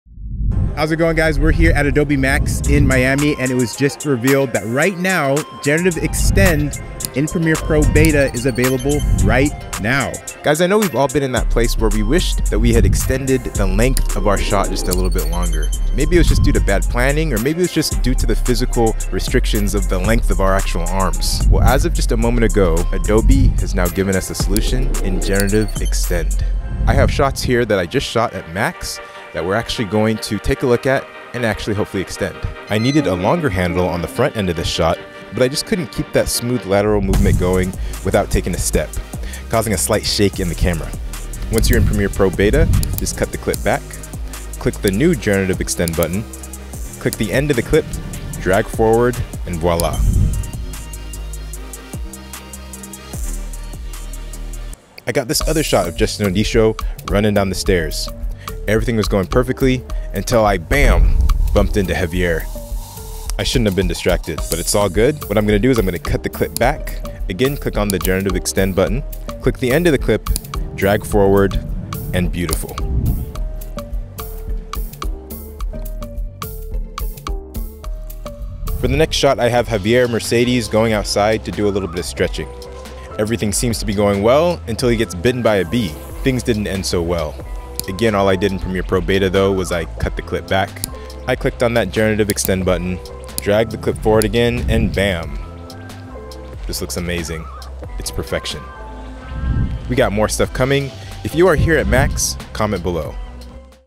NEW Premiere Pro Feature Is MORE Magical Than ⚡️ Harry Potter 🪄
Generative Extend in Premiere Pro is pretty impressive. Heres a really quick break down. 5 more videos on the way!